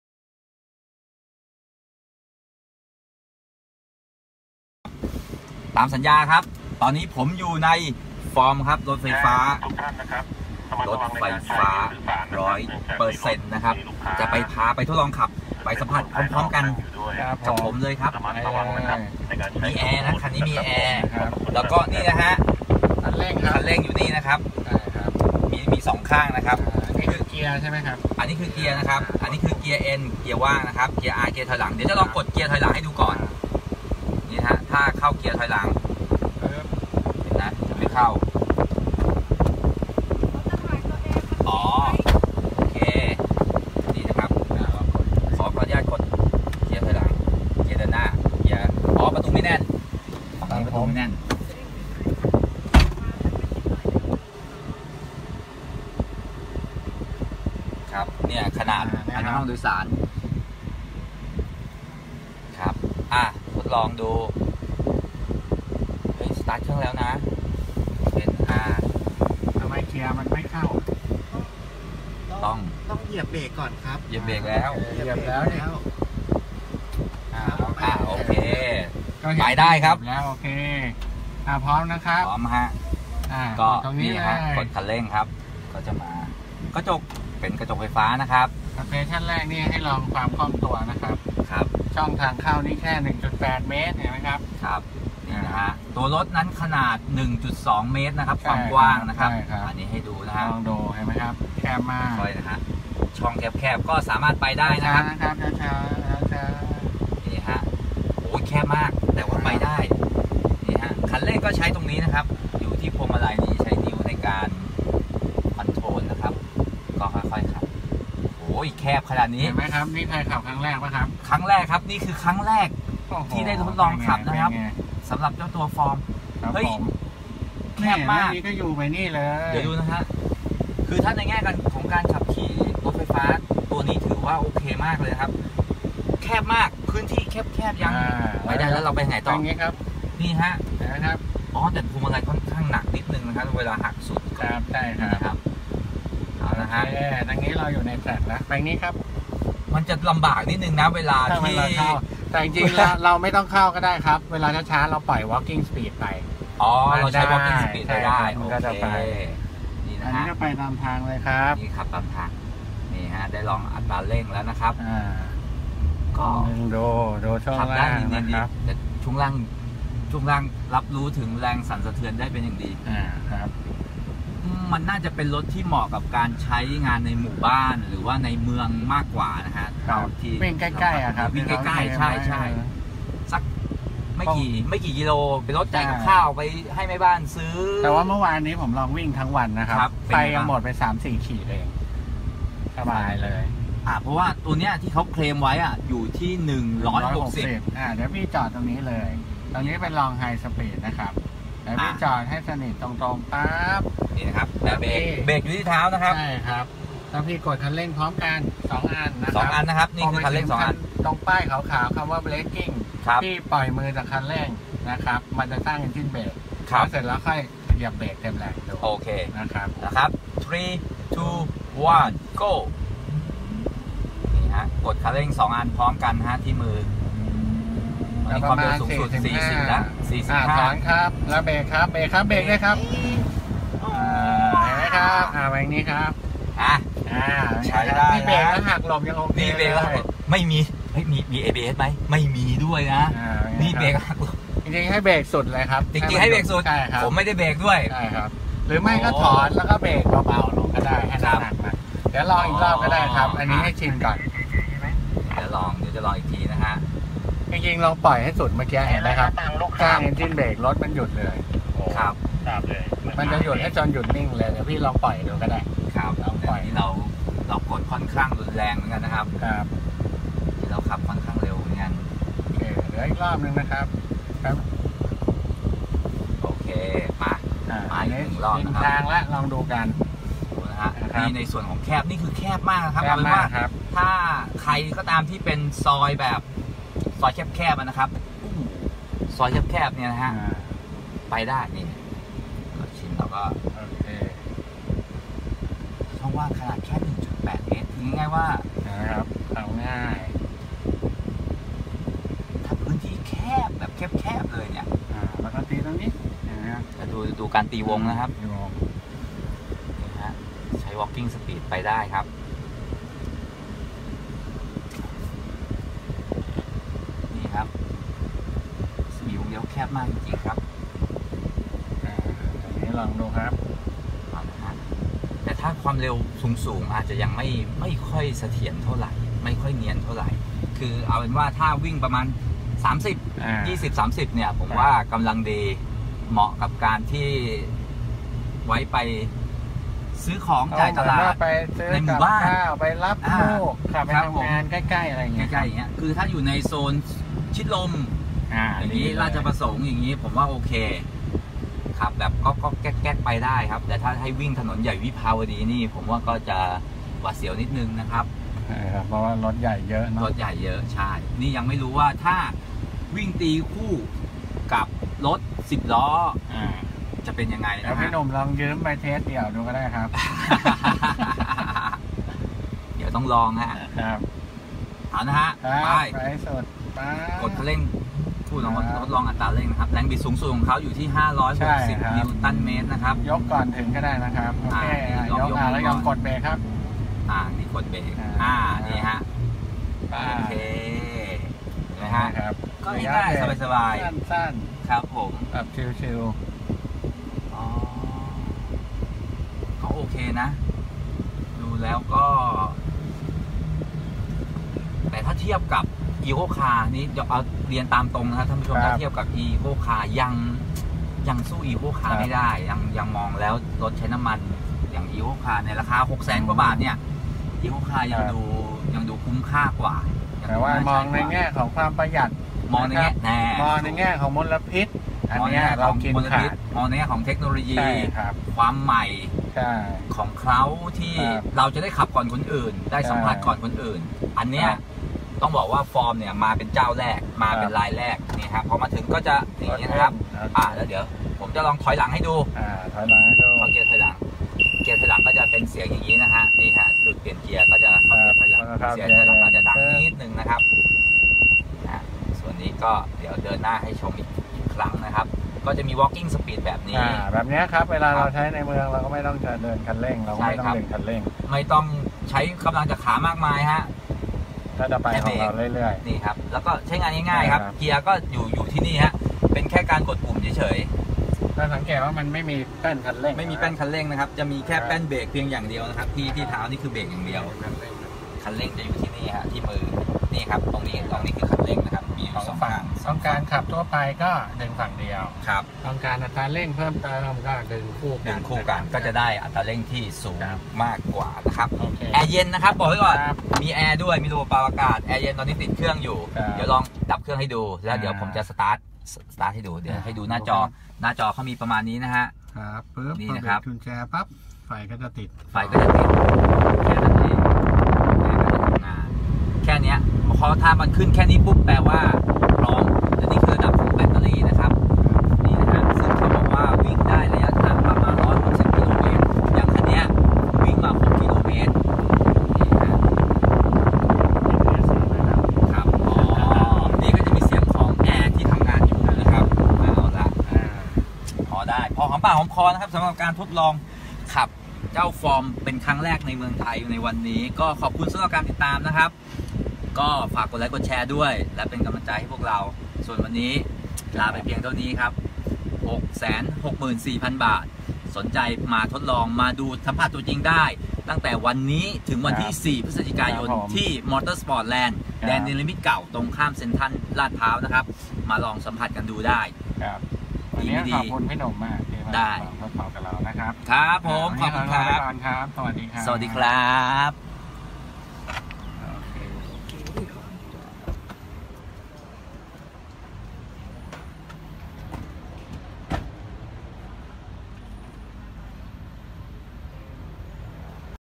ลองขับจริง FOMM ONE รถยนต์ไฟฟ้า น้องใหม่
2018/11/01 ลองขับจริง FOMM ONE รถยนต์ไฟฟ้า น้องใหม่

ลองขับจริง FOMM ONE รถยนต์ไฟฟ้า น้องใหม่ แรงแค่ไหน ขับเป็นอย่างไร กับค่าตัว 664,000 บาท พาไปลองกันจริงๆ...